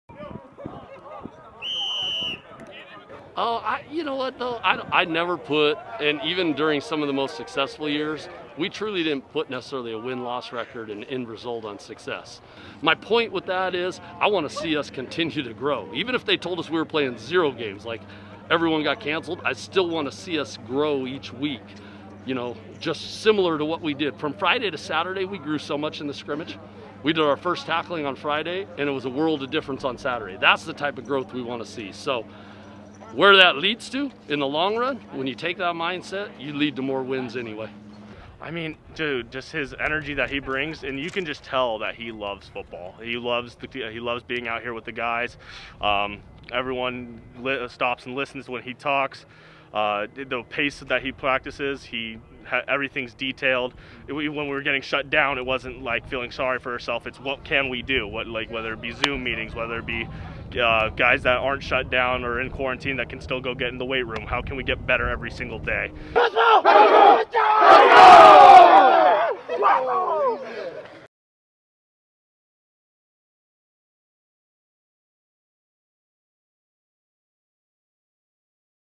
Cal Poly football prepares to open spring season
After a coronavirus imposed hiatus the Cal Poly football team and head coach Beau Baldwin are back on the field preparing for an abbreviated spring season for 2021.